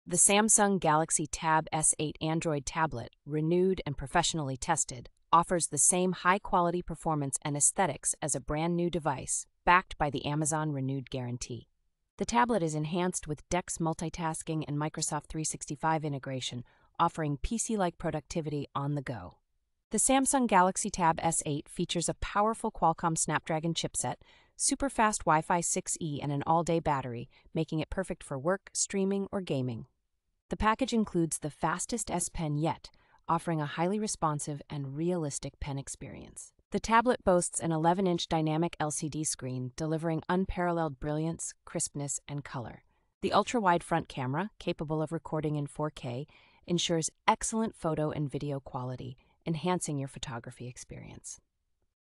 REVIEW: SAMSUNG Galaxy Tab S8 Tablet. ESSENTIAL details.
The SAMSUNG Galaxy Tab S8 Android Tablet, renewed and professionally tested, offers the same high-quality performance and aesthetics as a brand new device, backed by the Amazon Renewed Guarantee.

The tablet is enhanced with DeX Multitasking and Microsoft 365 integration, offering PC-like productivity on-the-go.

The SAMSUNG Galaxy Tab S8 features a powerful Qualcomm Snapdragon chipset, super fast WiFi 6E, and an all-day battery, making it perfect for work, streaming, or gaming.

The package includes the fastest S Pen yet, offering a highly responsive and realistic pen experience.

The tablet boasts an 11-inch dynamic LCD screen, delivering unparalleled brilliance, crispness, and color.

The ultra-wide front camera, capable of recording in 4K, ensures excellent photo and video quality, enhancing your photography experience.